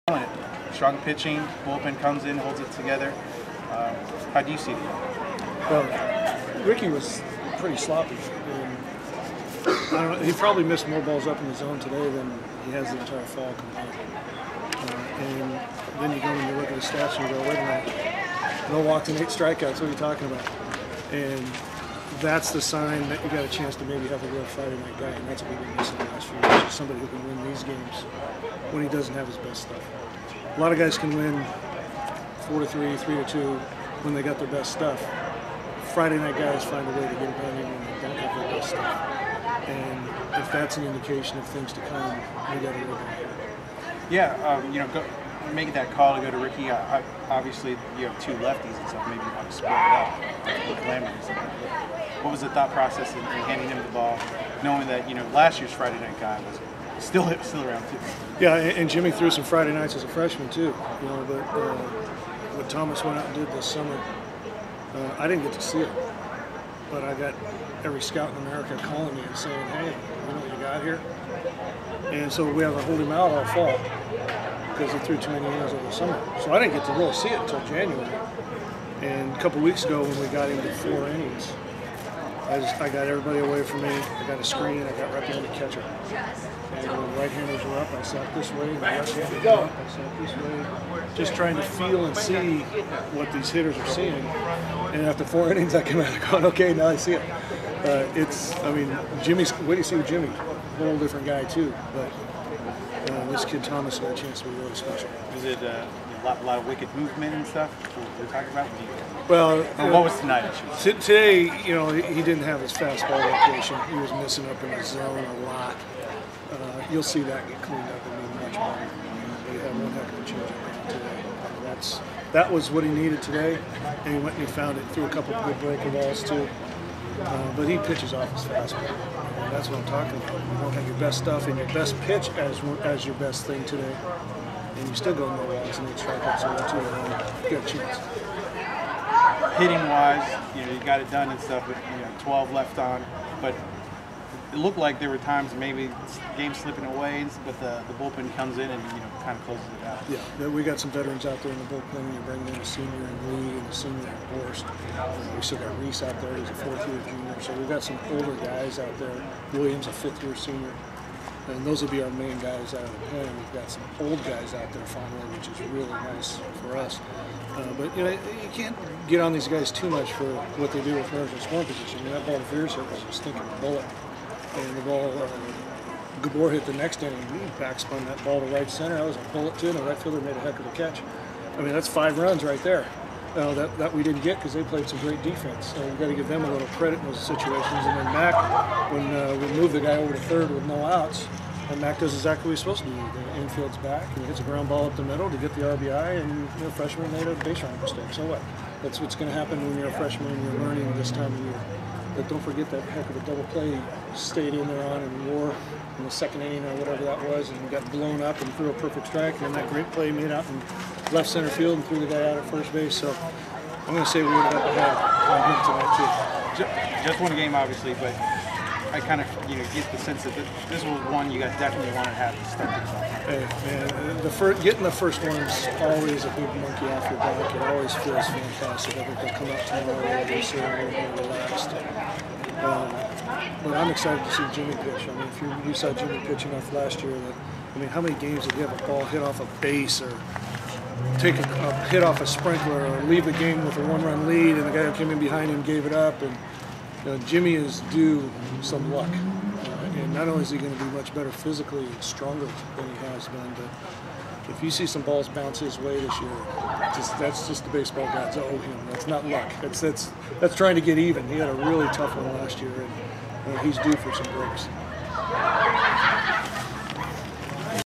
Head Coach Mike Batesole
Head Baseball Coach Mike Batesole speaks after the Bulldogs' 2-1 win over Creighton on Friday evening to start the 2016 season.